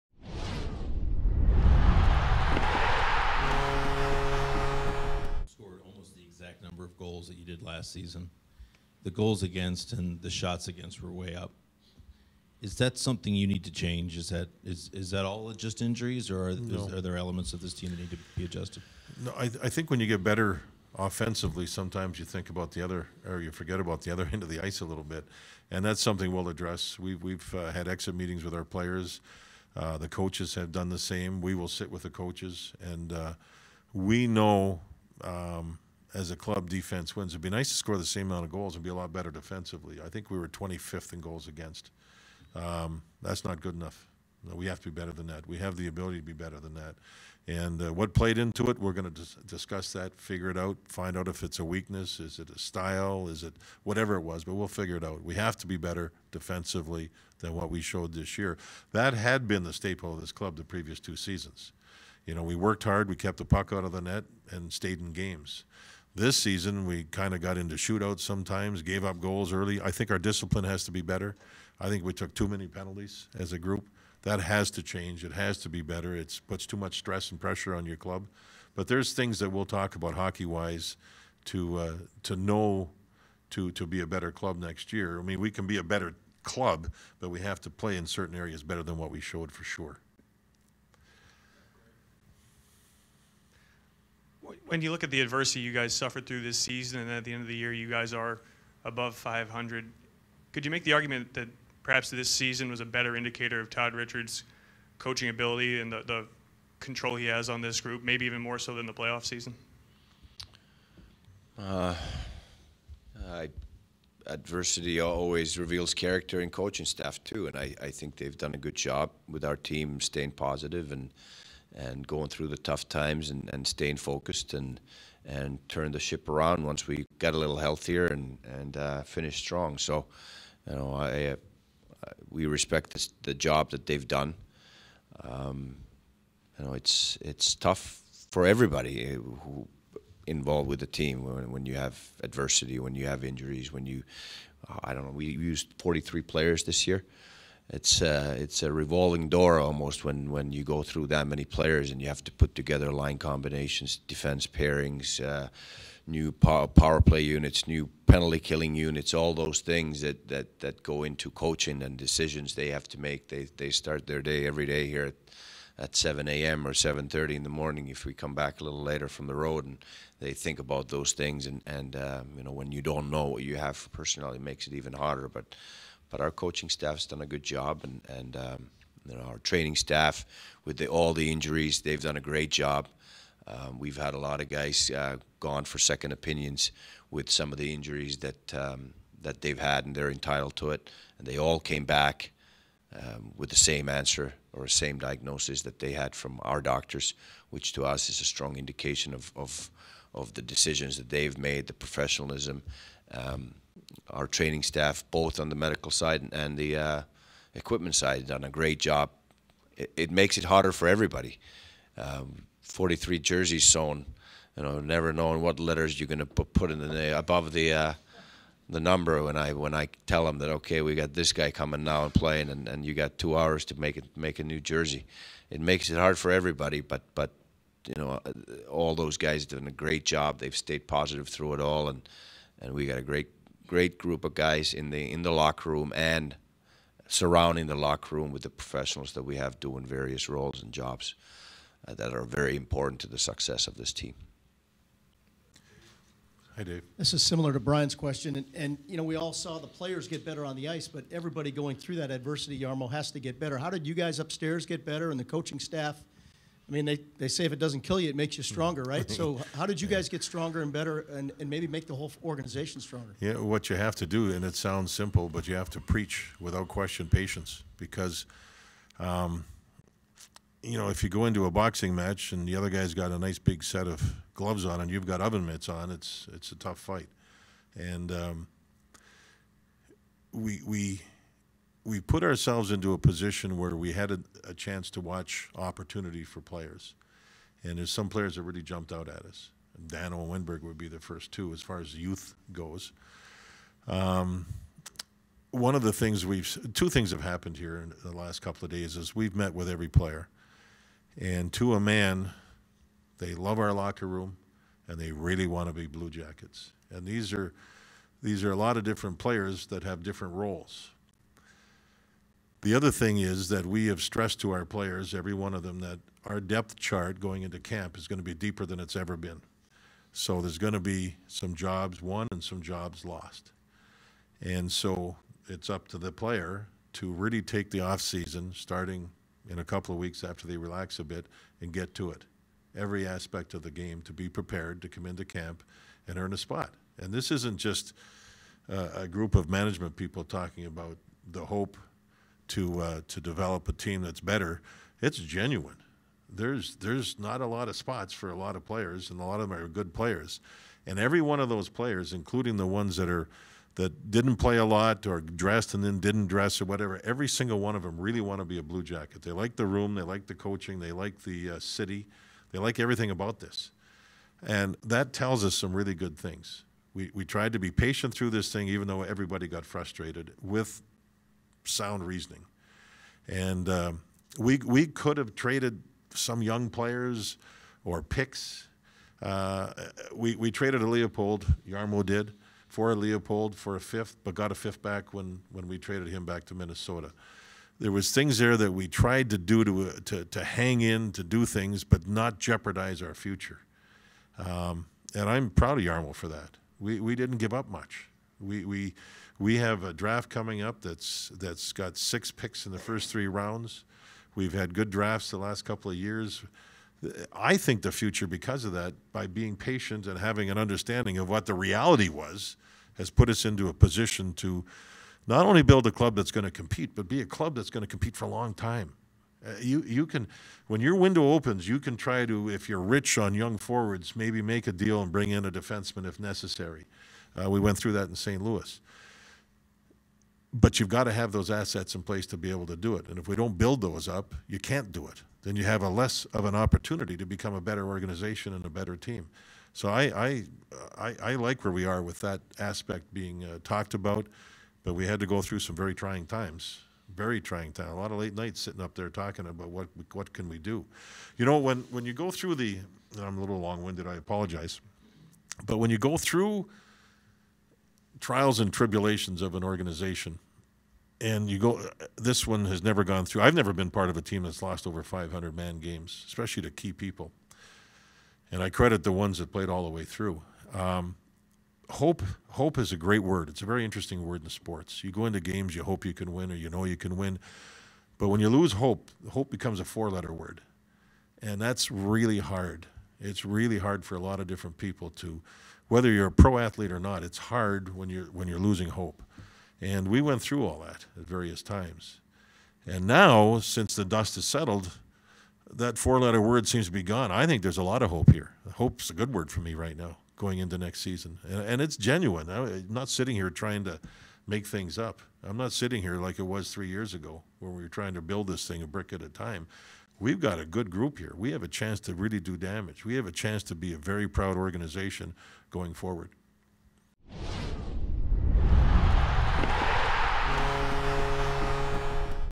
Exit Press Conference (4/13/15): John Davidson and Jarmo Kekalainen Part 2
JD and Jarmo talk about the style of play, overcoming adversity and the team's depth next season during their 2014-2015 season exit press conference.